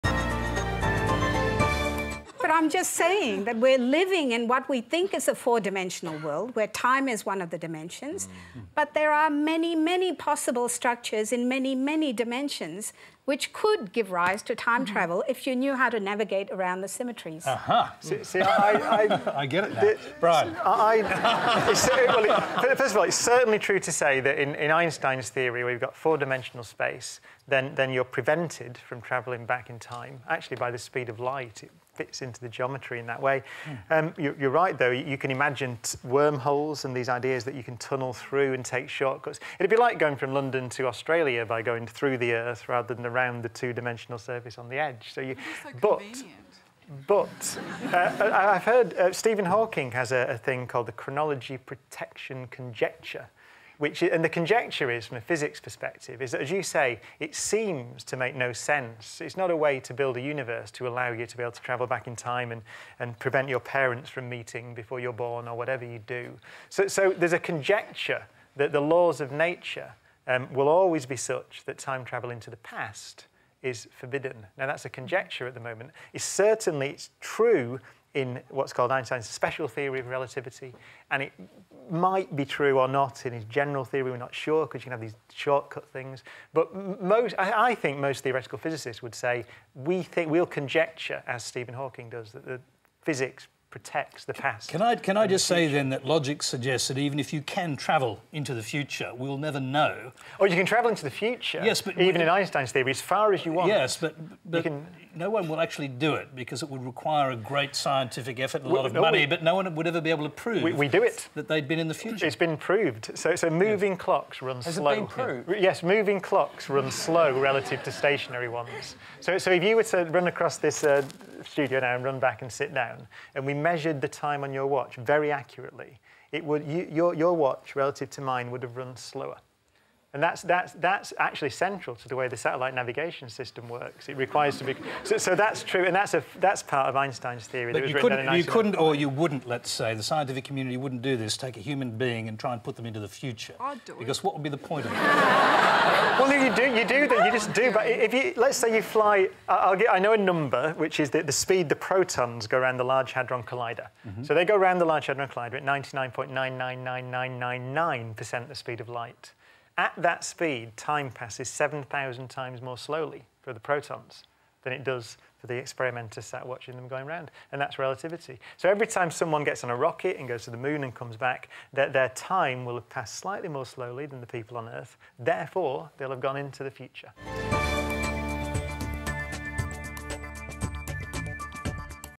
Q&A Highlight – Physics Protects The Past
Professor Brian Cox explains the conjecture that time travel into the past is forbidden however time travel into the future has been proven.